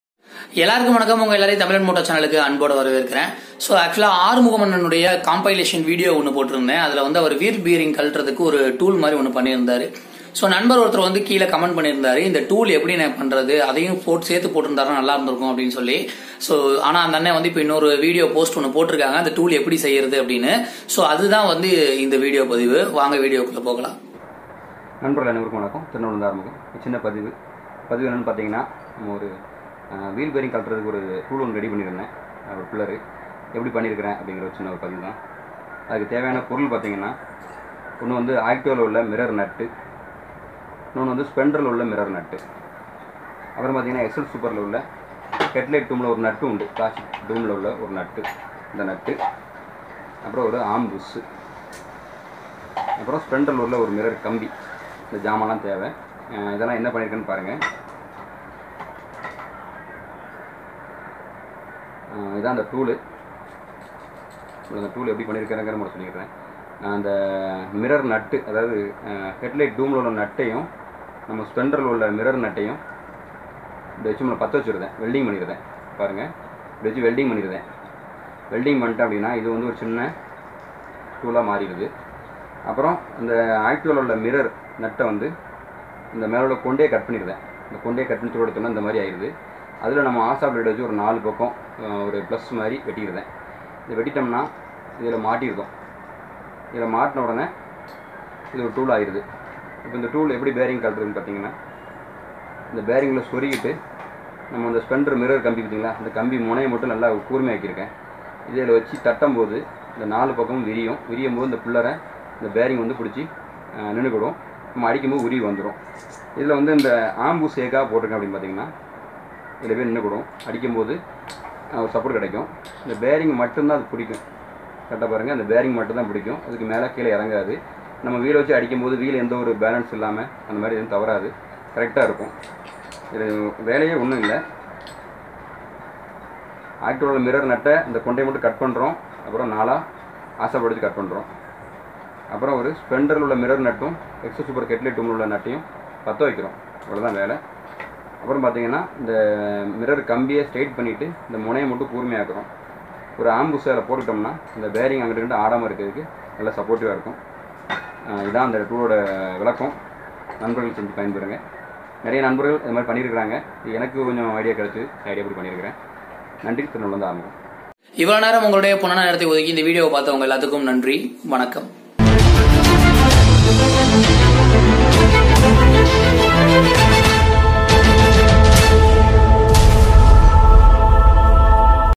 Wheel bearing removing tool making video from Mechanic Arumugam anna | Tamilanmoto
This video describe that Wheel bearing tool making video from Mechanic Arumugam anna | Tamilanmoto

Arumugam anna compilation video